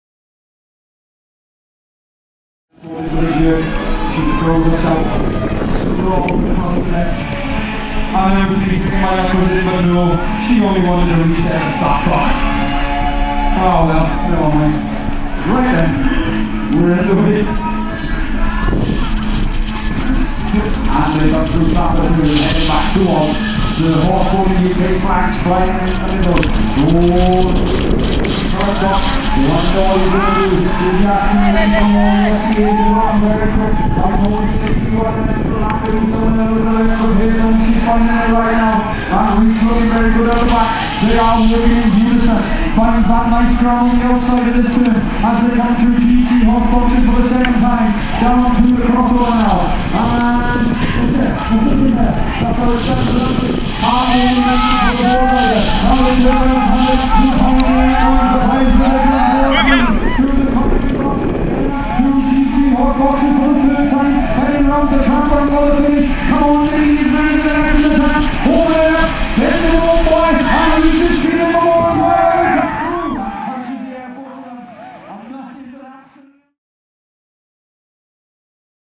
Horseboarding UK ,Scally Wags, Round 1, Thorsby Hall
Watch the Scally Wags fastest run from Thorsby from both perspectives!